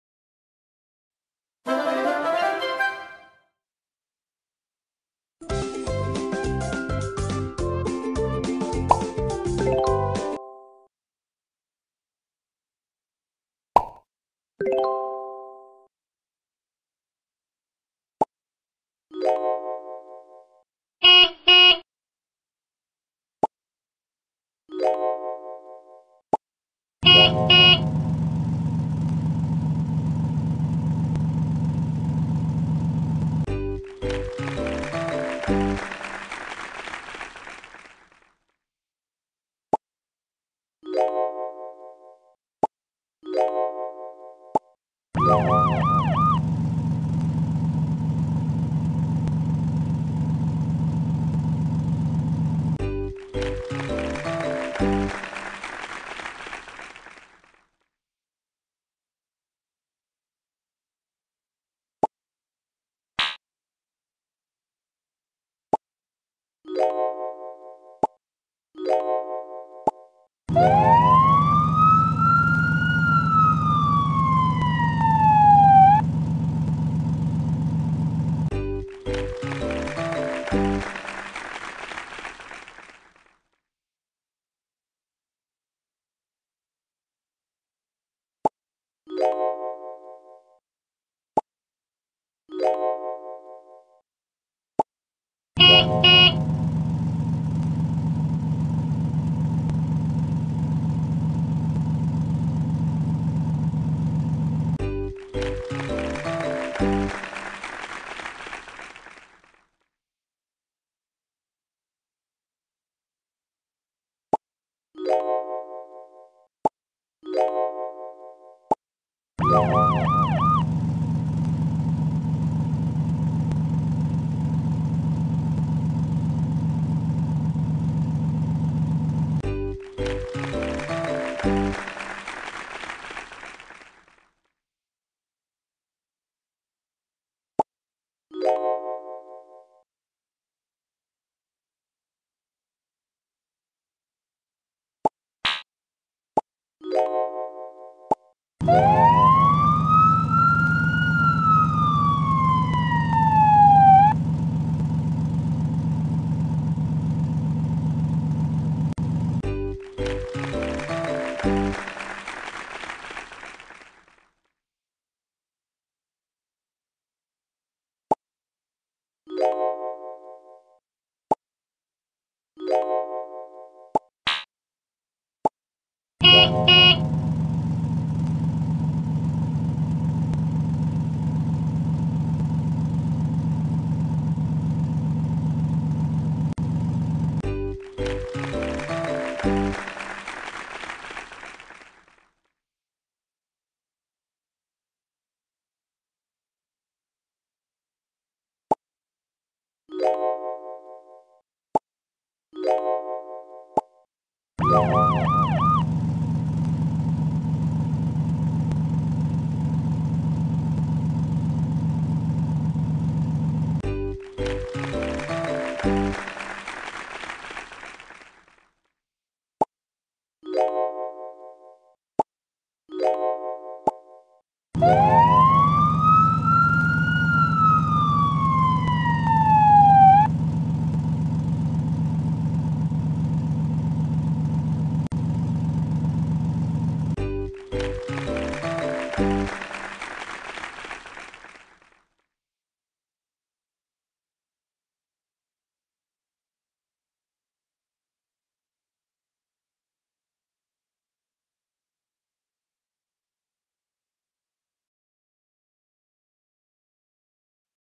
bermain melengkapi lintasan
video kali ini bermain melengkapi lintasan agar mobil mobil bisa berjalan dengan baik .selamat menonton